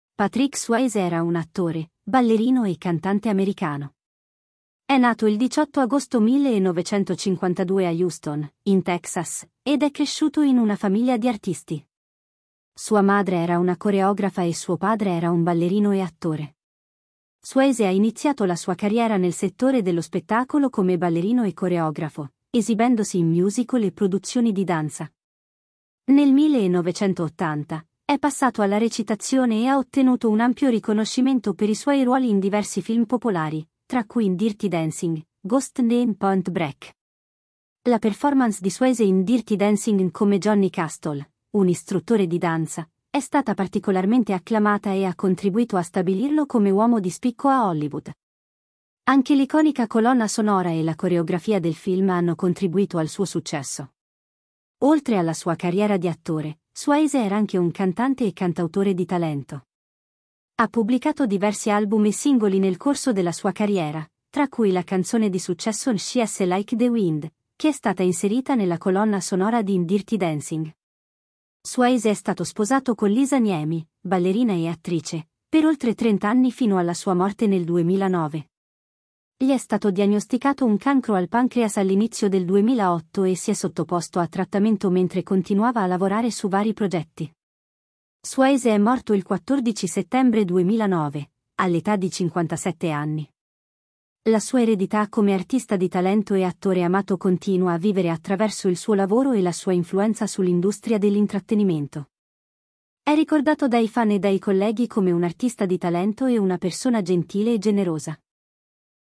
La biografia di Patrick Swayze morto 14 settembre 2009
Patrick Swayze era un attore, ballerino e cantante americano che ha guadagnato fama per i suoi ruoli in diversi film popolari negli anni 1980 e 1990. È nato il 18 agosto 1952 a Houston, in Texas, ed è cresciuto in una famiglia di artisti.Purtroppo, a Swayze è stato diagnosticato un cancro al pancreas nel 2008 ed è morto il 14 settembre 2009, all'età di 57 anni. È stato ricordato dai fan e dai colleghi come un artista di talento e una persona gentile e generosa.